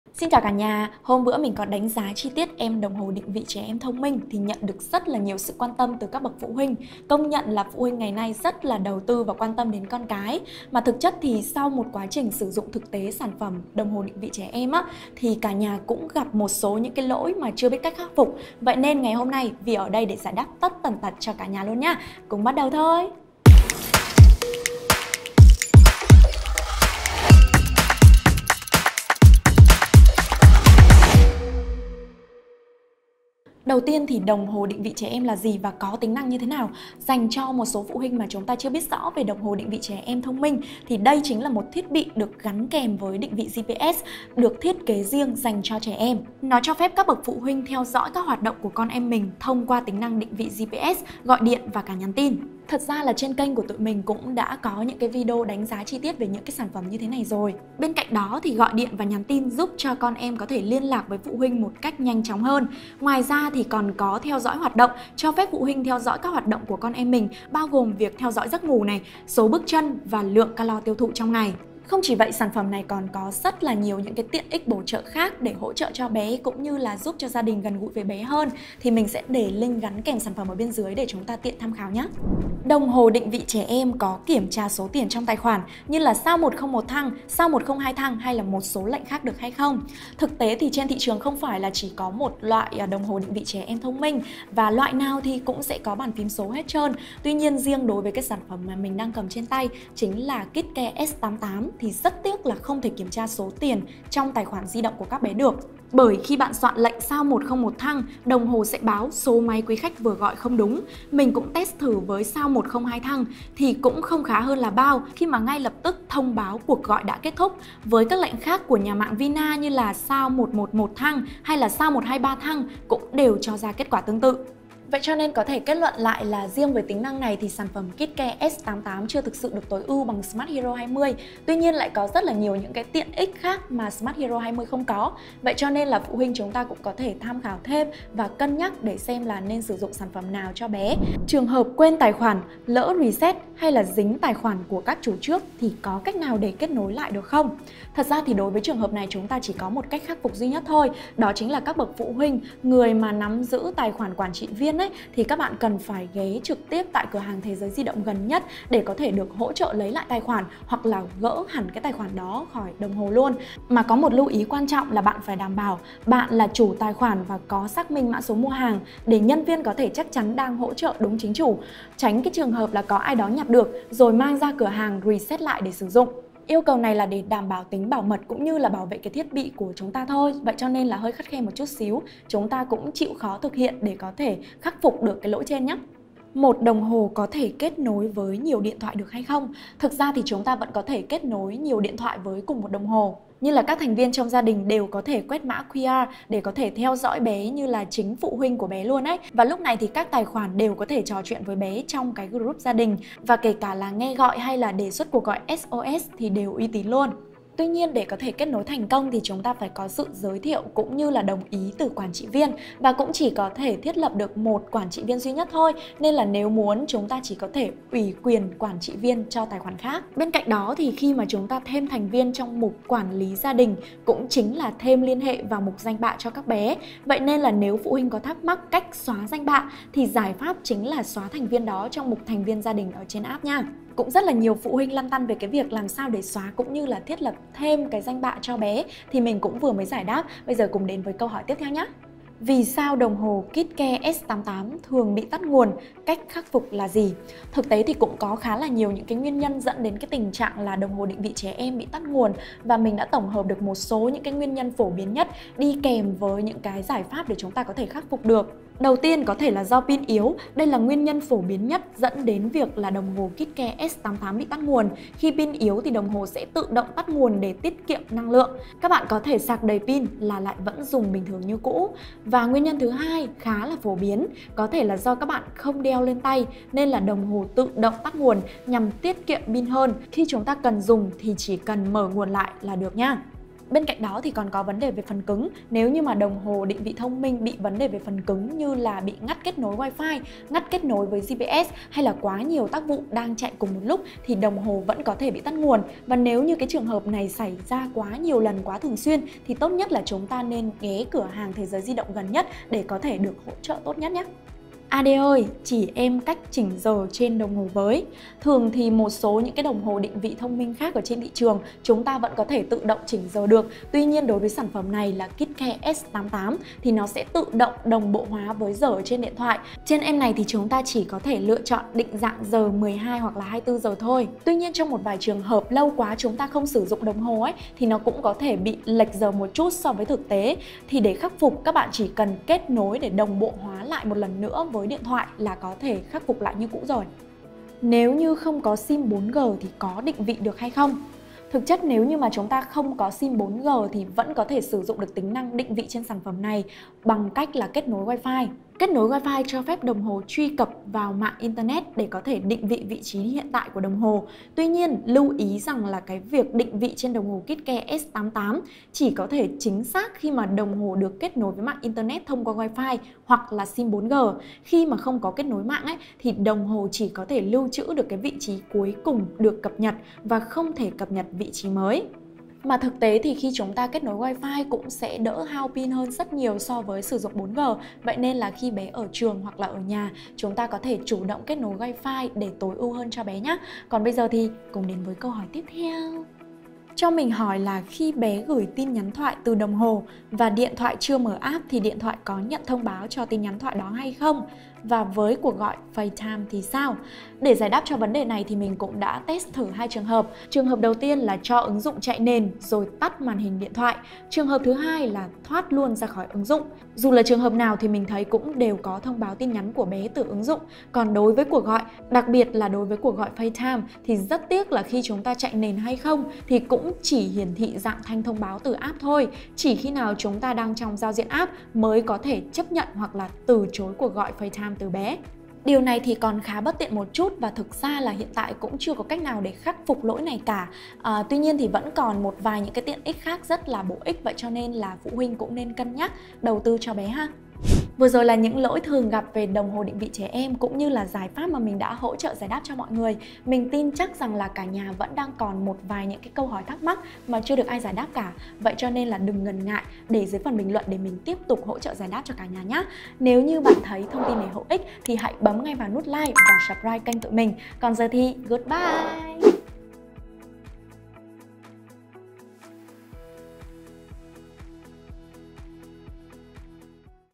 Những câu hỏi về "lỗi" của Đồng Hồ Định Vị Trẻ Em, phụ huynh NHẤT ĐỊNH PHẢI XEM! | Thế Giới Đồng Hồ
Đồng hồ định vị trẻ em thông minh là một thiết bị giúp cho các phụ huynh có thể dễ dàng chăm sóc và theo dõi các bé tốt hơn. Thế nhưng vẫn còn tồn tại một số những "lỗi" mà phu huynh chưa biết cách khắc phục. Hãy xem hết video này để cùng mình giải đáp nhé!

00:00 - Mở đầu
00:33 - Giới thiệu về đồng hồ trẻ em
01:23 - Đồng hồ định vị trẻ em là gì và có tính năng như thế nào?
03:10 - Một đồng hồ có thể kết nối với nhiều điện thoại được không?
04:03 - Vì sao đồng hồ Kidcare S88 thường bị tắt nguồn? Cách khắc phục là gì?
05:05 - Cách chỉnh giờ trên đồng hồ?
05:40 - Nếu không có sim 4G có định vị được không?
06:28 - Khi bé gửi tin nhắn thoại từ đồng hồ và điện thoại chưa mở App thi điện thoại có nhận thông báo cho tin nhắn thoại đó không? Và với cuộc gọi Facetime thì sao?
07:18 - Tổng kết